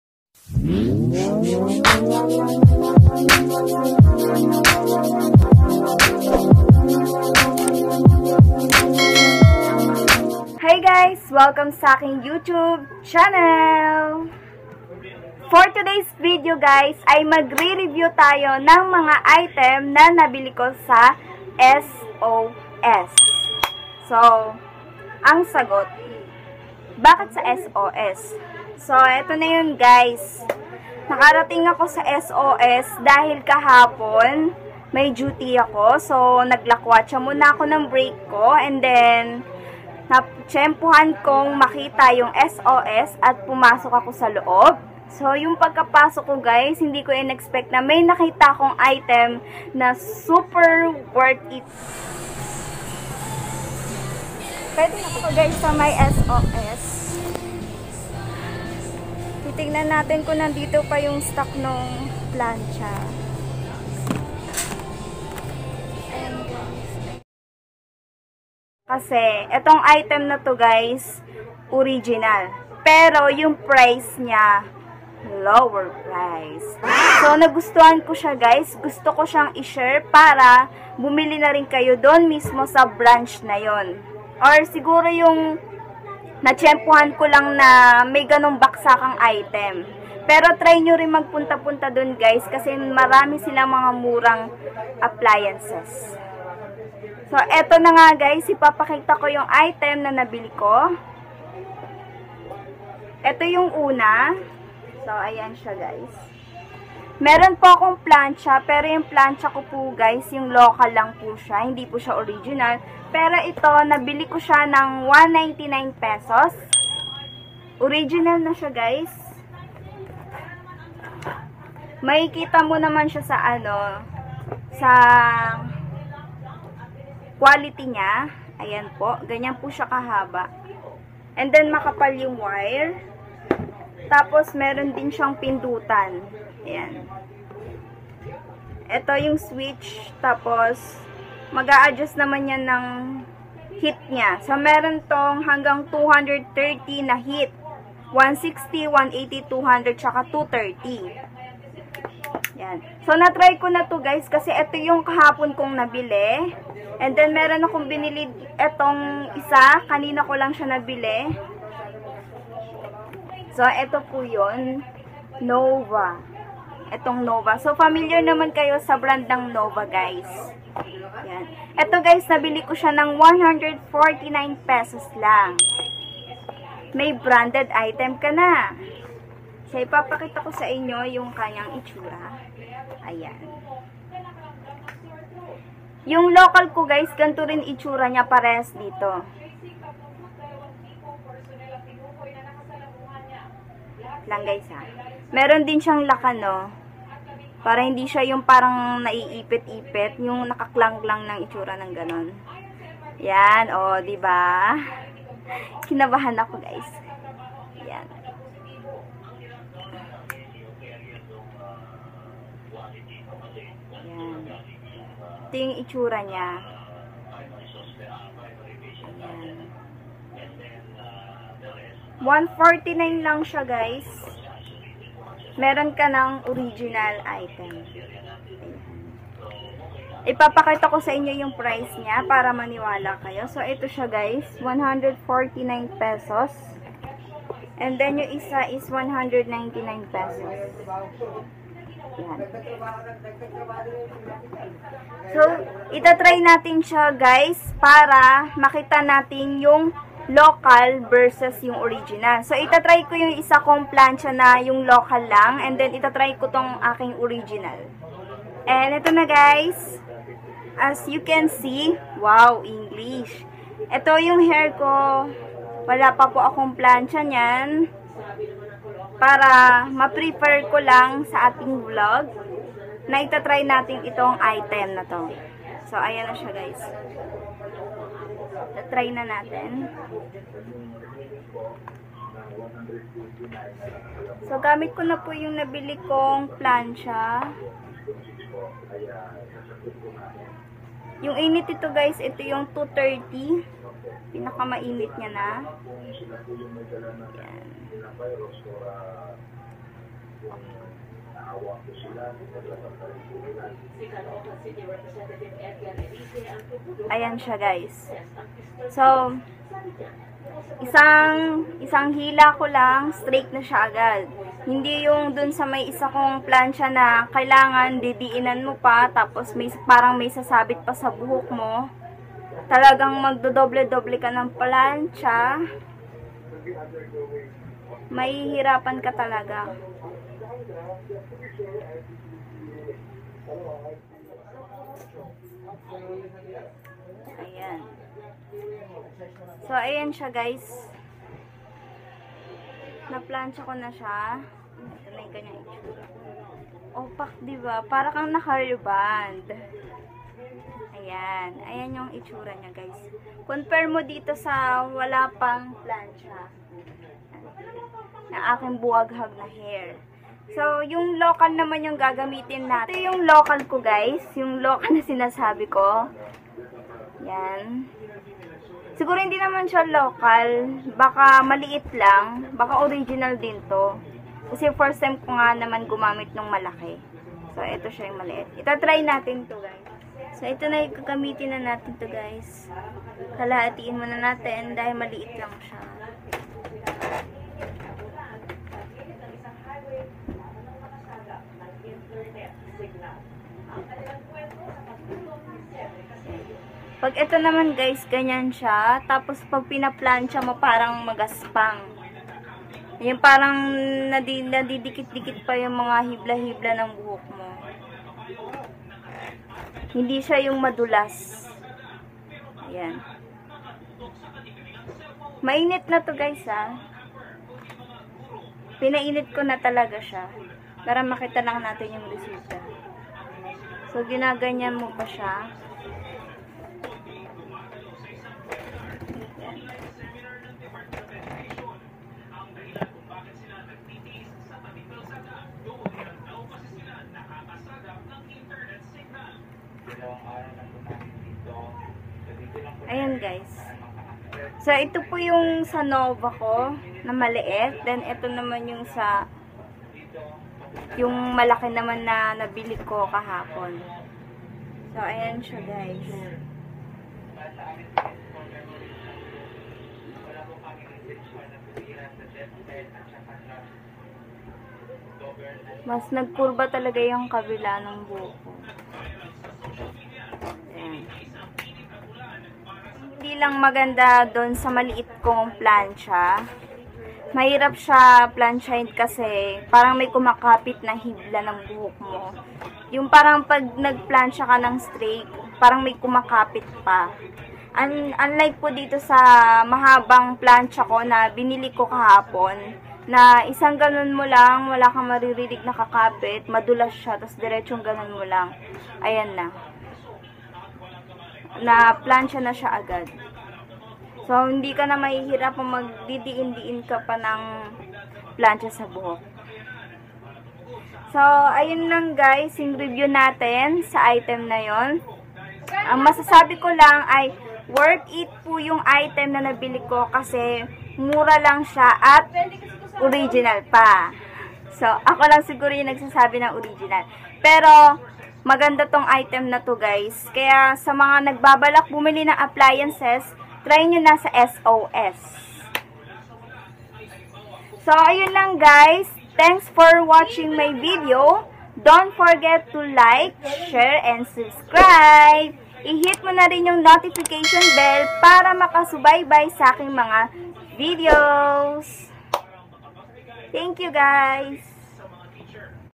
Murang Flat iron?? Super Worth it ! (Hair straightener)
For this video mag rereview tayo ng hair straightener" na nabili ko sa SOS for only 199 pesos!  Oo 199 lang 😍😍 ang ganda ng quality nya kaya dalaws narin ang nabili ko , before yan wag mo kalimutan mag like share and subscribe! i hit mo narin yung notification bell! Para maka follow ka sa mga next kong video 🤗🤗 Love you all guys! 😘